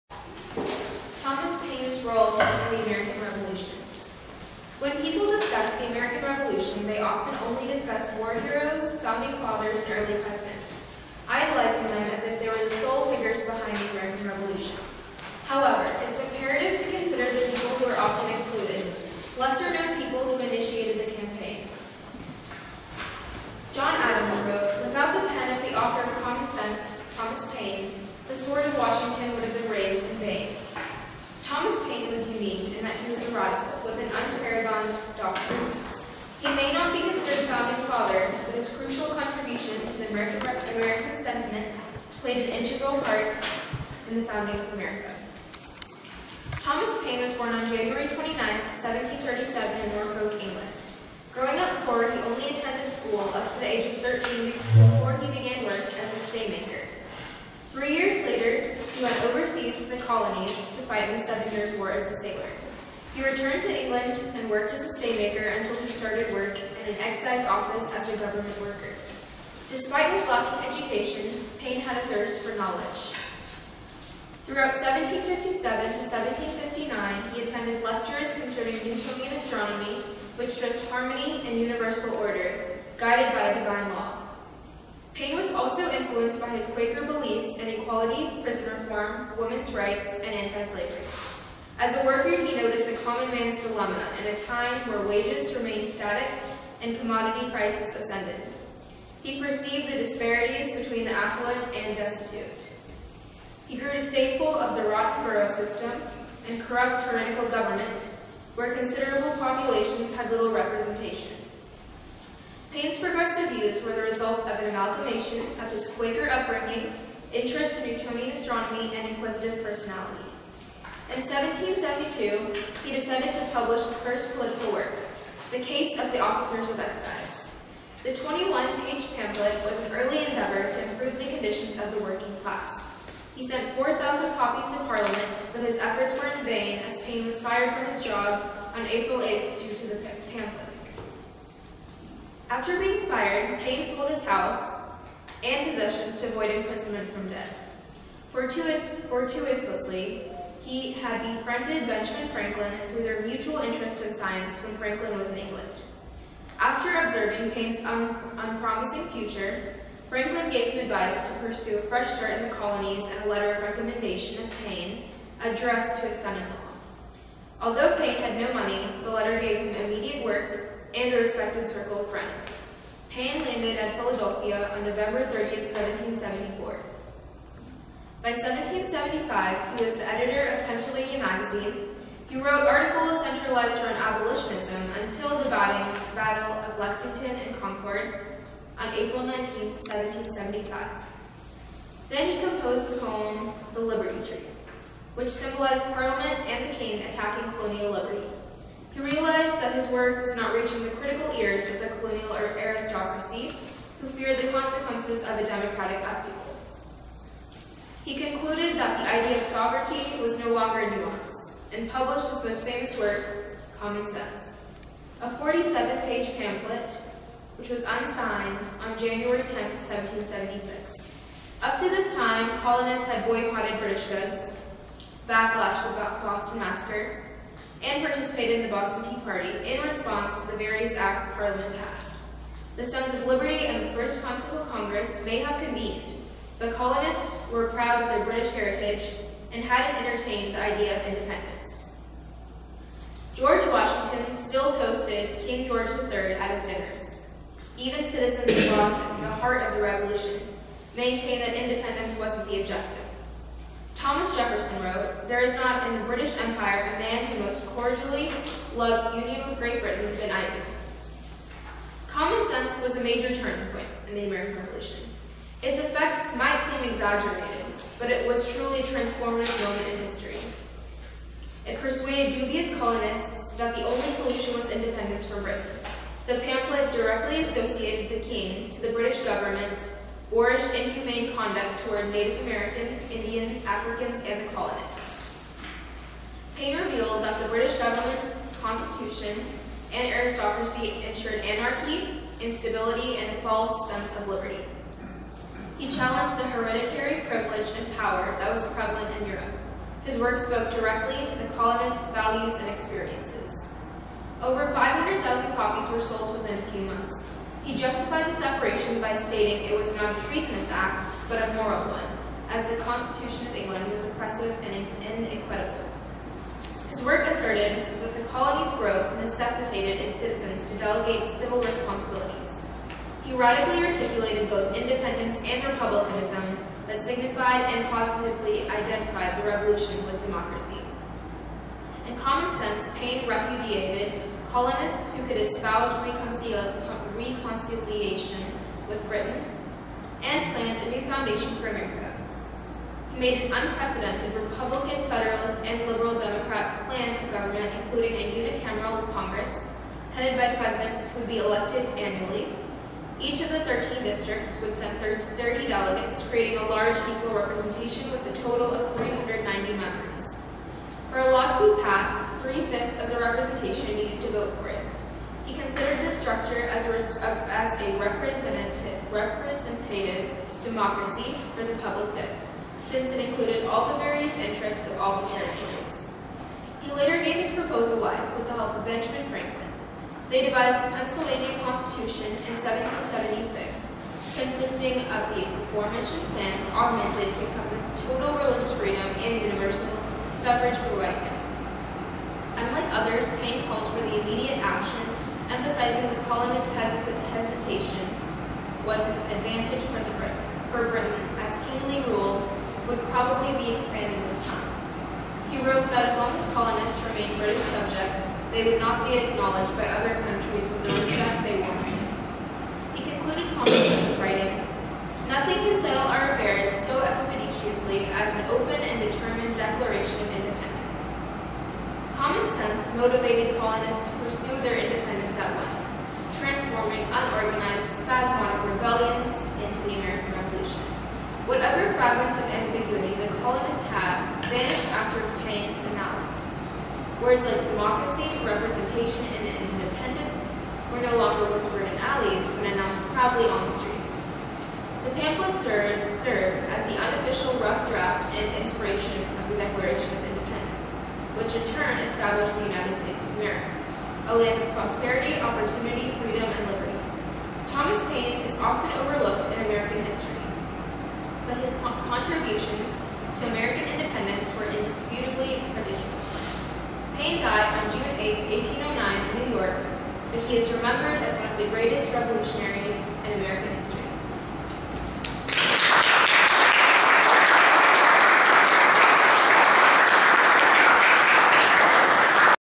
Thomas Paine Presentation
Presented By:
Nikhithi (Nikki) Lavu
During the Sons of the American Revolution Quarterly Meeting